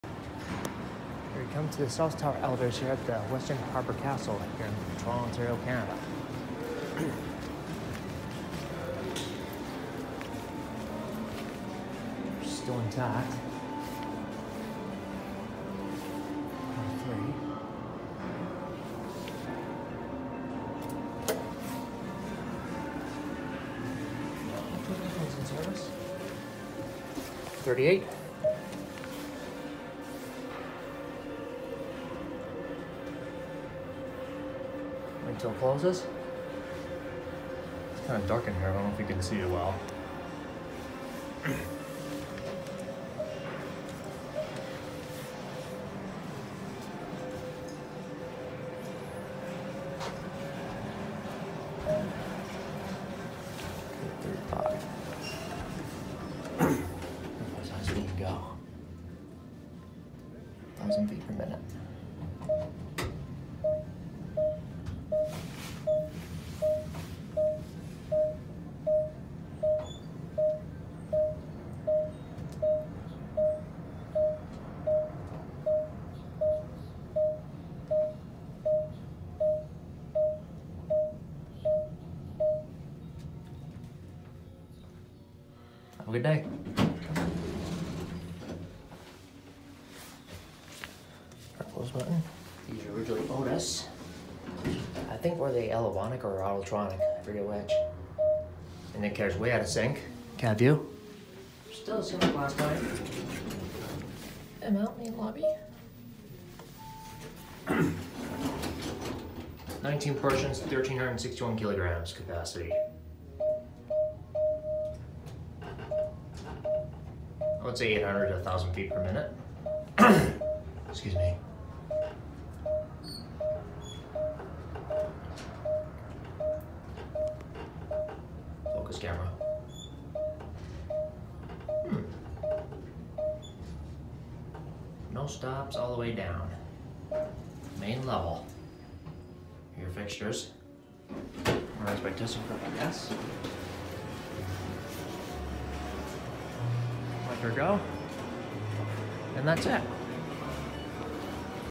OTIS High-Speed South Tower Elevators at the Westin Harbour Castle Toronto, ON (60FPS Retake)
This was filmed yesterday at around 8:49am during another EPIC Uplifting Adventure in Toronto!

I was close by coming back from the Hotel X by Exhibition Place so...
Why not ride and film these in 1080p but at 60FPS?!?

Also, Marriott now owns the Westin Hotels chain!!! Wow! Didn’t know that until I found out when I was here!